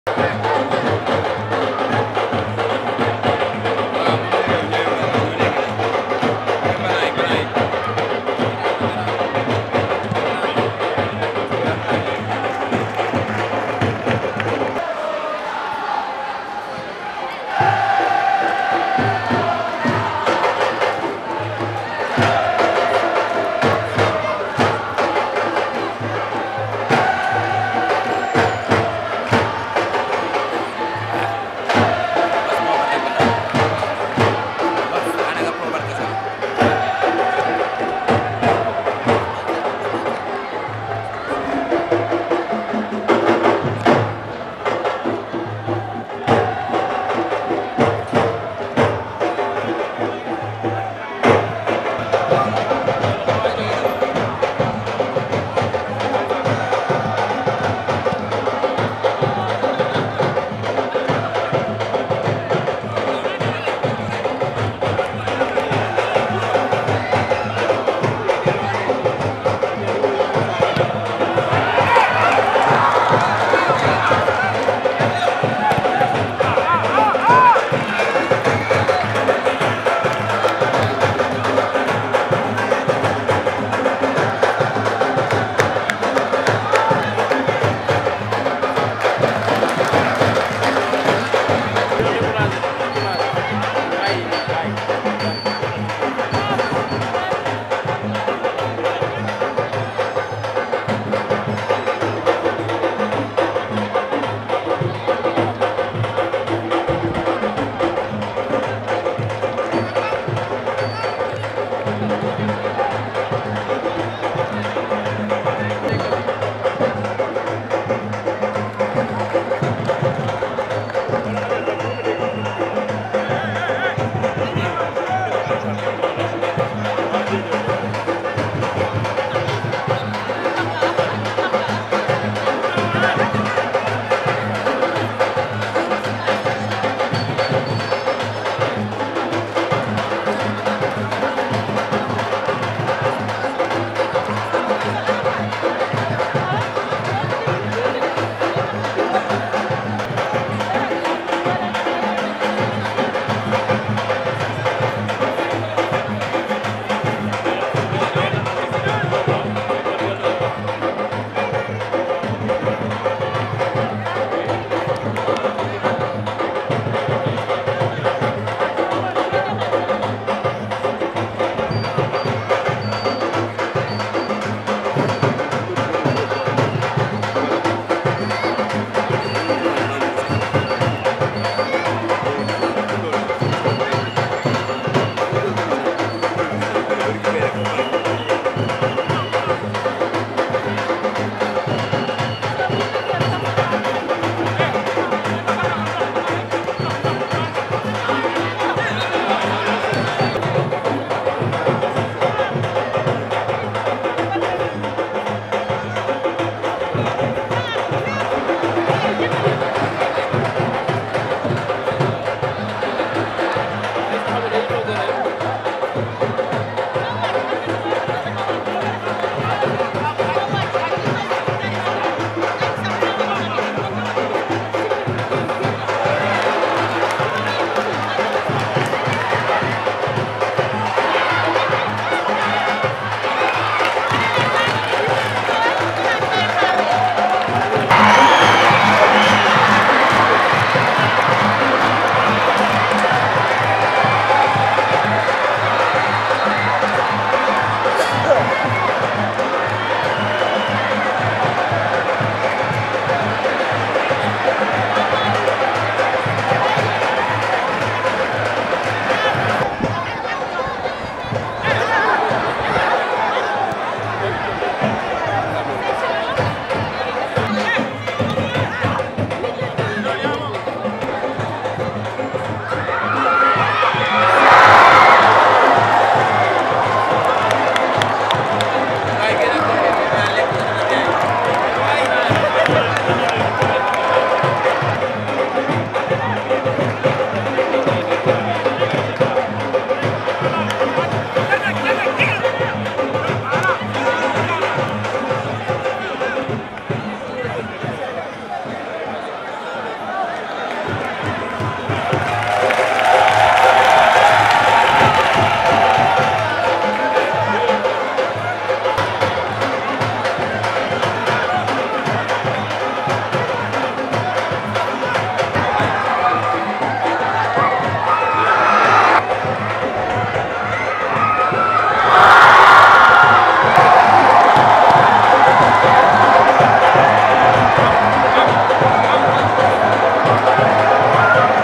NAVETANE RUFISQUE 2021 : RESUME QUART DE FINAL MOOM SA REW 1 VS TERANGA 0
RUFISQUE SENEGAL TV LA TELE DE LA JEUNESSE